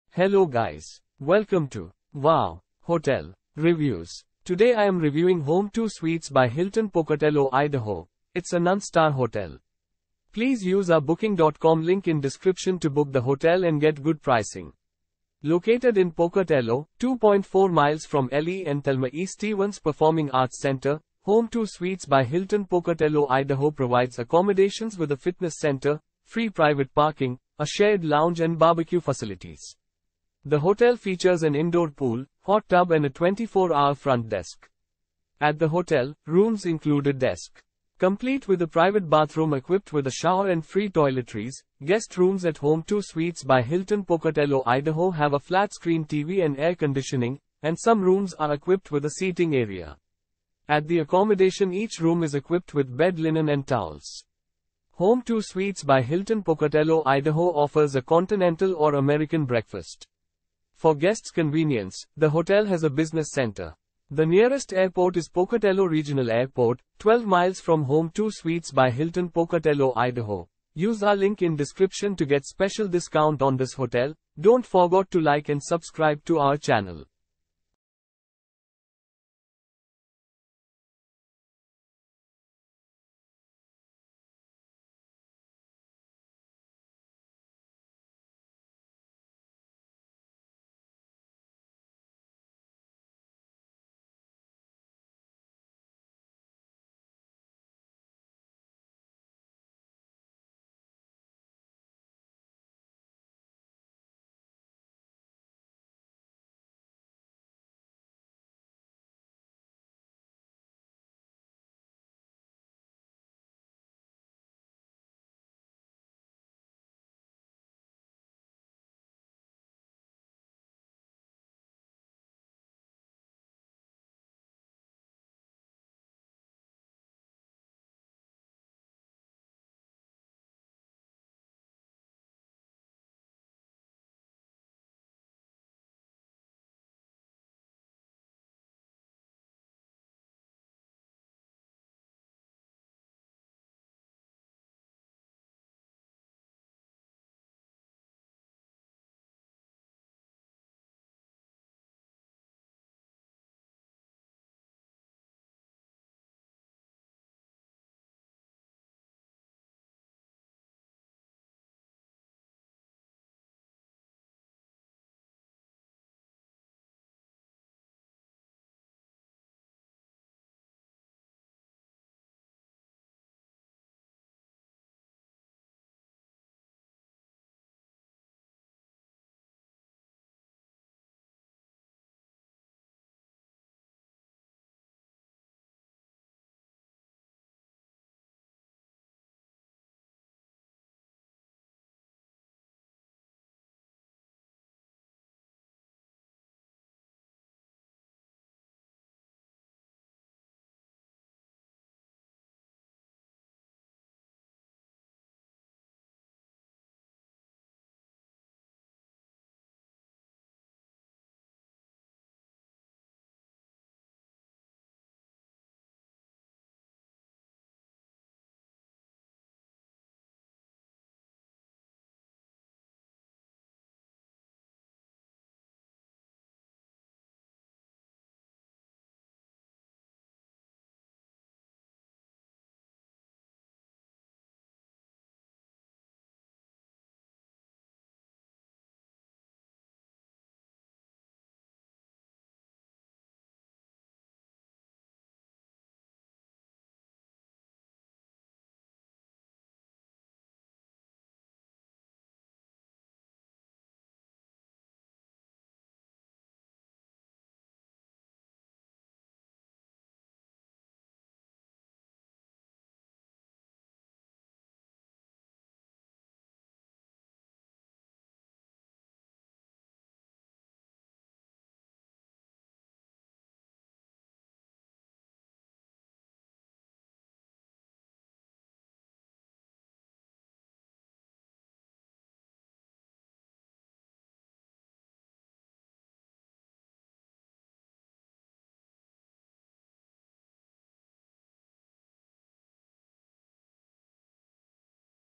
Home2 Suites By Hilton Pocatello, Id Review - Pocatello , United States of America
Home2 Suites By Hilton Pocatello, Id

Located in Pocatello, 2.4 miles from L.E. And Thelma E. Stephens Performing Arts Center, Home2 Suites By Hilton Pocatello, Id provides accommodations with a fitness center, free private parking, a shared lounge and barbecue facilities. The hotel features an indoor pool, hot tub and a 24-hour front desk.

At the hotel, rooms include a desk. Complete with a private bathroom equipped with a shower and free toiletries, guest rooms at Home2 Suites By Hilton Pocatello, Id have a flat-screen TV and air conditioning, and some rooms are equipped with a seating area. At the accommodation each room is equipped with bed linen and towels.

Home2 Suites By Hilton Pocatello, Id offers a continental or American breakfast.

For guests' convenience, the hotel has a business center.

The nearest airport is Pocatello Regional Airport, 12 miles from Home2 Suites By Hilton Pocatello, Id.